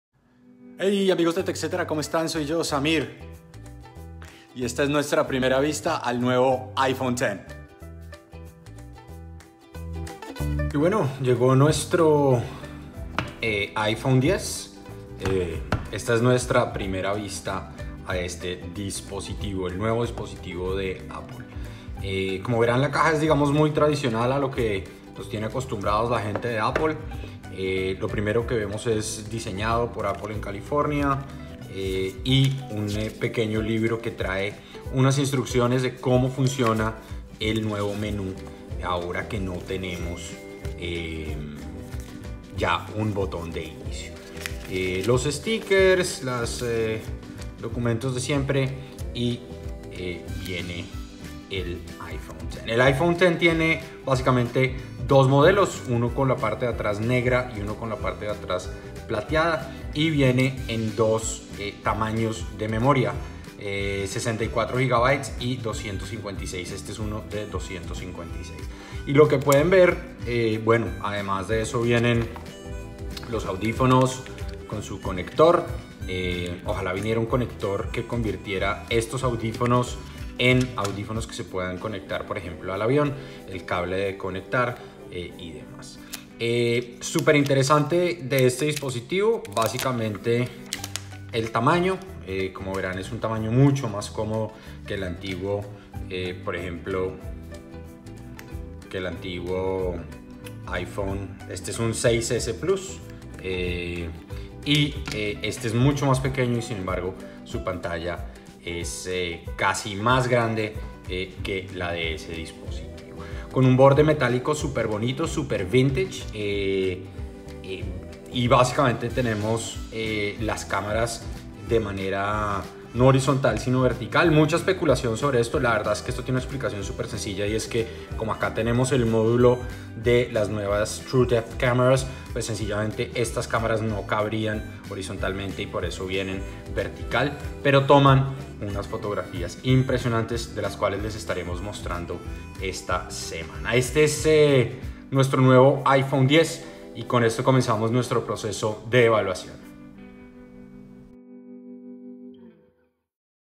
Este es nuestro Unboxing del iPhone X - TECHcetera
Llegó nuestro iPhone X y aquí está nuestro unboxing

TECHcetera es un canal donde encontraras reseñas y consejos. La tecnología al alcance de tu mano con un lenguaje sencillo.